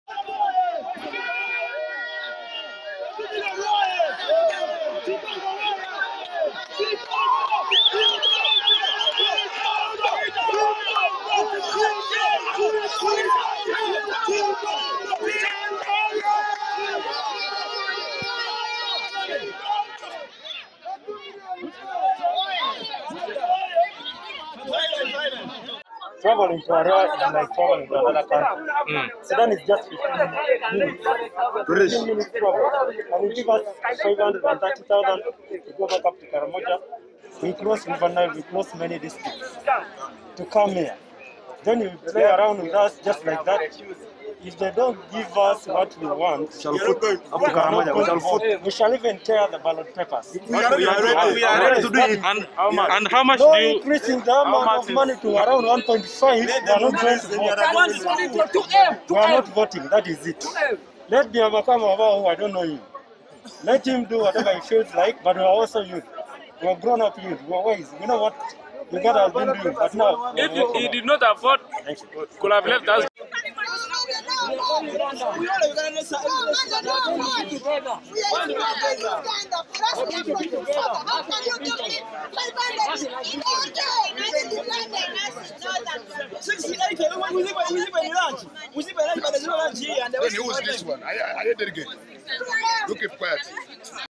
Chaos delays youth MP polls in Arua
Chaos broke out among the youth who had gathered to elect their MP on Friday at Mvara Senior Secondary school.The chaotic scenes and back and forth disagreements among the youths and Electoral Commission officials then delayed the electoral process.The delegates accused the Electoral Commission for allegedly reducing the amount of money they are to receive as their allowance for accommodation, feeding and transport. The over 443 youth delegates are demanding to be paid Shs 2 million each.